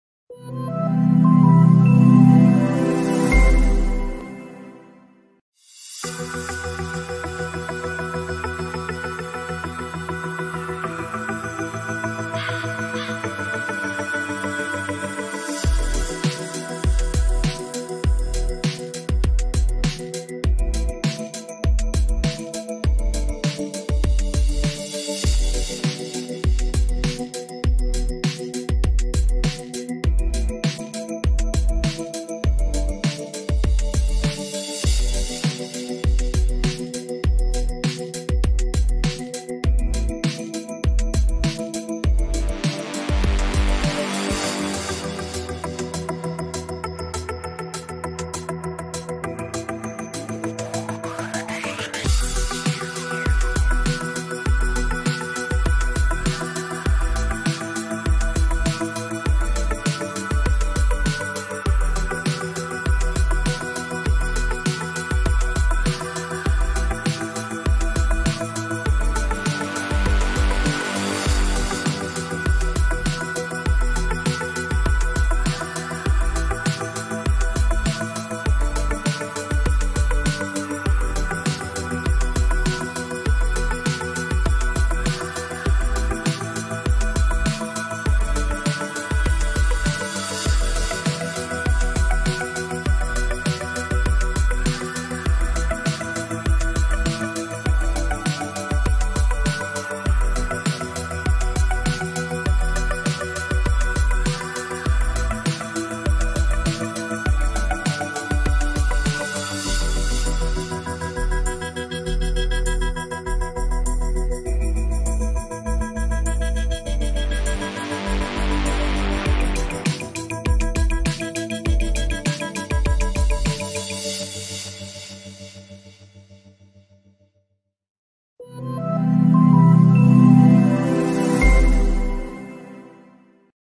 "Sovelmash" construction site / Day seven
During the week, almost daily, we have been posting new videos from the construction site, where the construction of the building frame for the "Sovelmash" D&E is underway at a rapid pace. The installation of metal structures and casting the concrete foundations continue, staircases and elevator shafts are being formed, as well as much more.